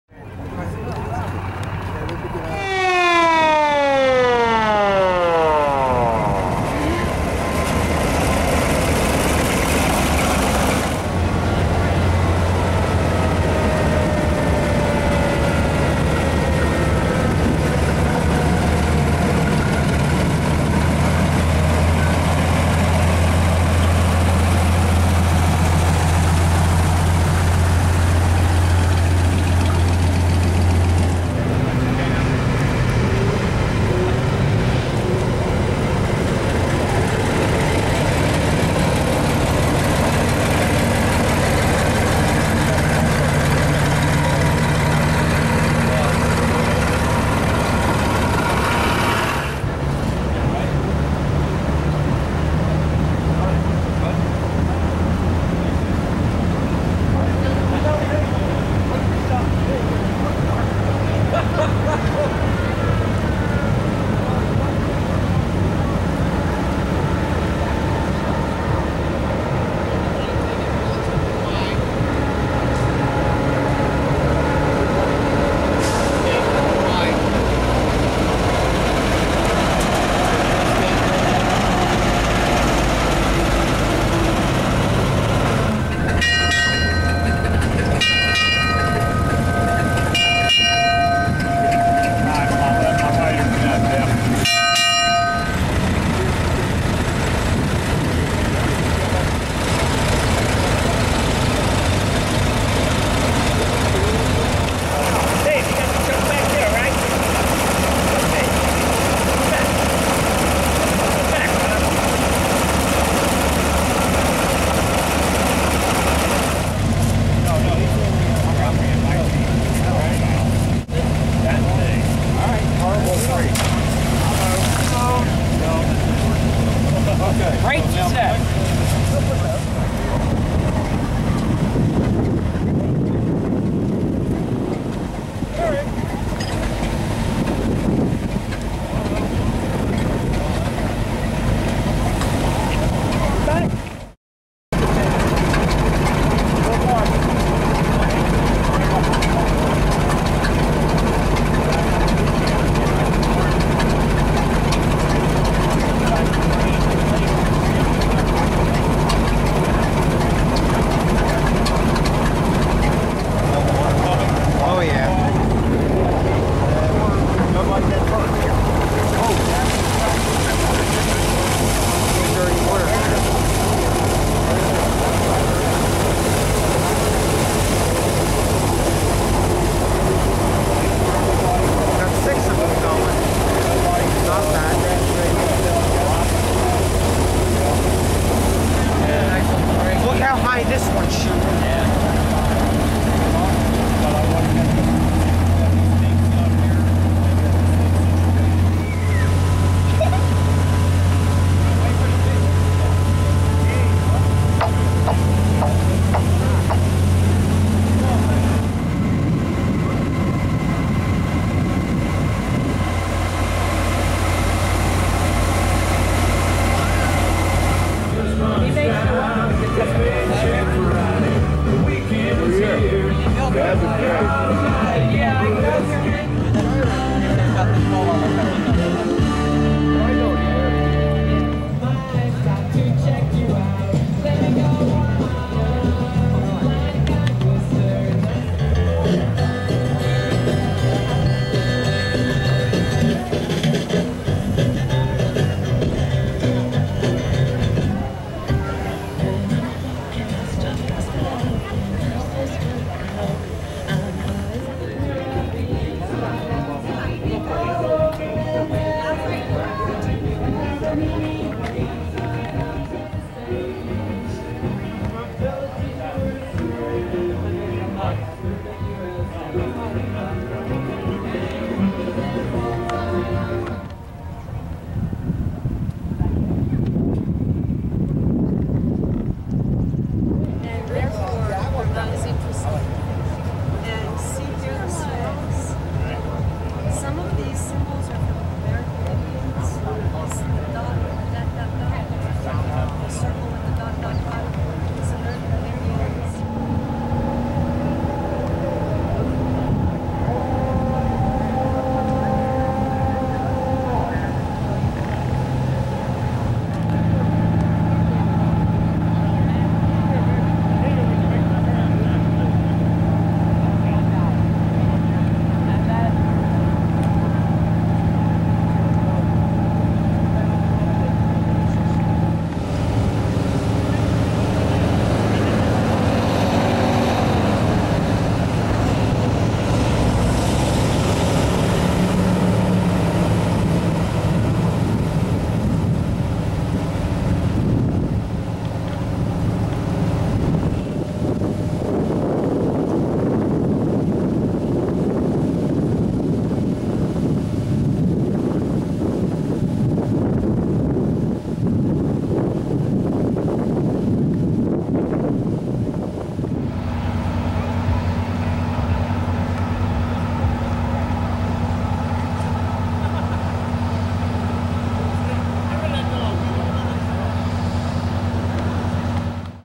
JAMESBURG NEW JERSEY FIRE PARADE AND MUSTER 6/21/14
The Tri-Counties Fire Association 23rd Annual Fire Apparatus Show and Flea Market was held in Thompson Park in Jamesburg New Jersey. This year Ahrens Fox rigs were featured.